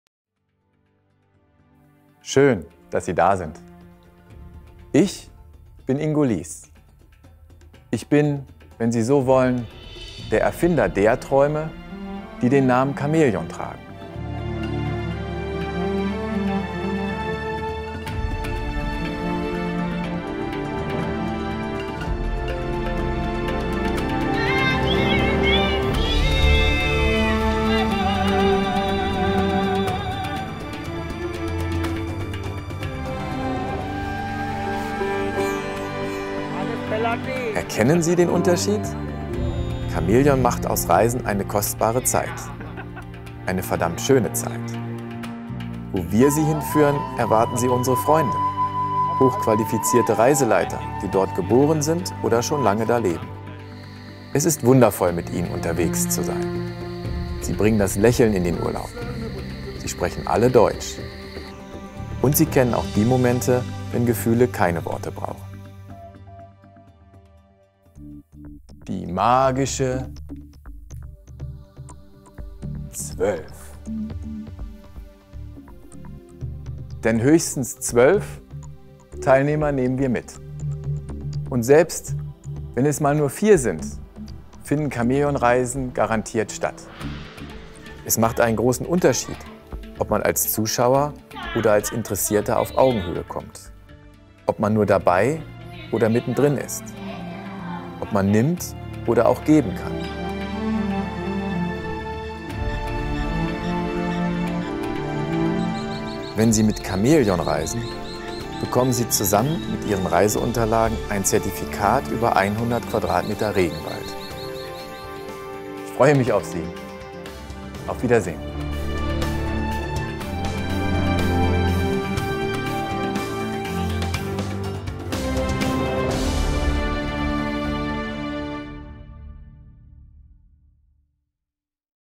Unsere Art des Reisens (Trailer)
Wir zeigen Ihnen unsere Art des Reisen und transportieren in überwältigenden Bildern die Botschaft zwischen den Zeilen: Träume nicht dein Leben, sondern lebe deinen Traum. Chamäleon -- Die Reise Ihres Lebens.

Den kompletten Film auf DVD, Kataloge und mehr gibt es in Ihrem Reisebüro oder